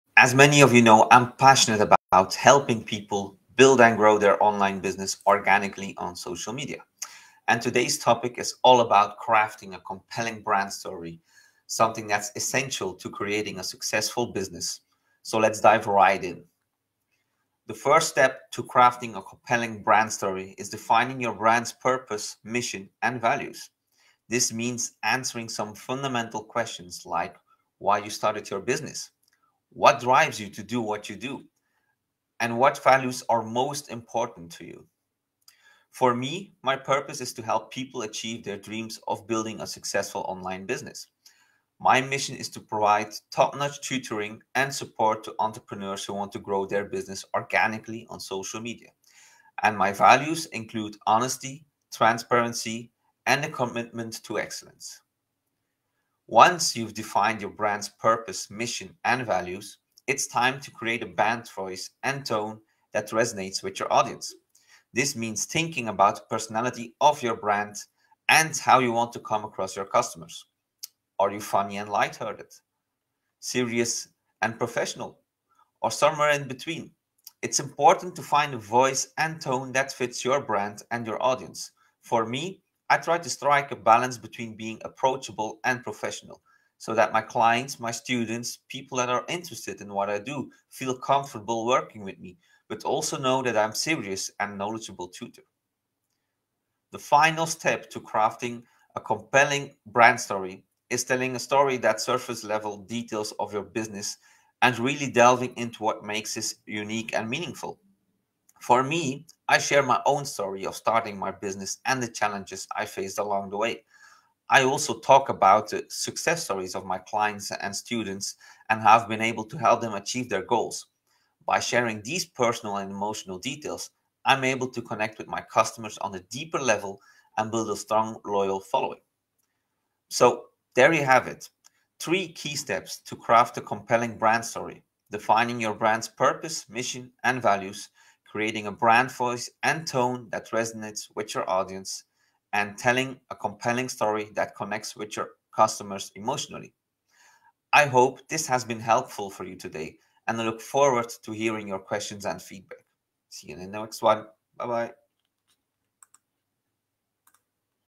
The Ultimate Guide to Crafting a Brand Story That Sells: Expert Tips and Secrets Revealed!
Ready to take your business to the next level?
Want to create a brand story that resonates with your customers and drives sales?
In this can't-miss live, I'll share my expert tips and secrets on how to craft a compelling brand story.
You'll learn how to define your brand's purpose, mission, and values, create a voice and tone that connects with your audience, and tell a story that will make them fall in love with your brand.
Don't miss out on this game-changing opportunity!